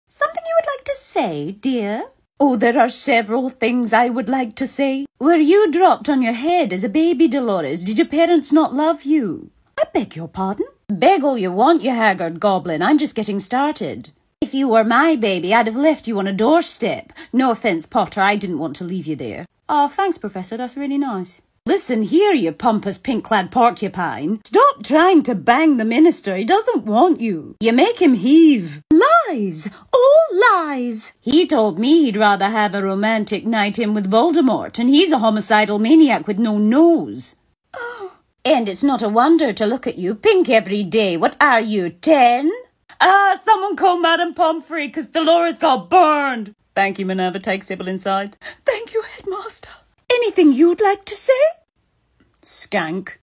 || McGonagall VS Umbridge || Gacha Club || Inspired By: @LittleKiwi || READ DESC ||
Sorry this is bad, it was rushed and i had no time literally. The OC is terrible ik, it only has Minnie and Harry tho. I might remake it cuz its horrible, but thx for watching my lil muffins, bye!